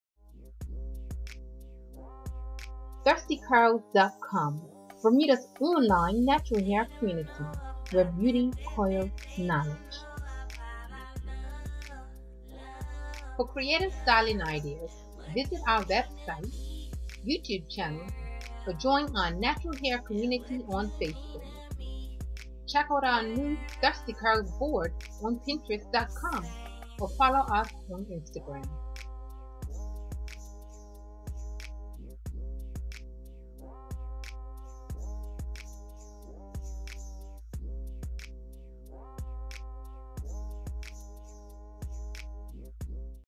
Thirsty Curlz commercial
Thirsty Curlz celebrates the beauty of black women and natural hairstyles.  It's were beauty coils knowledge
Facebook.com/Thirstycurlz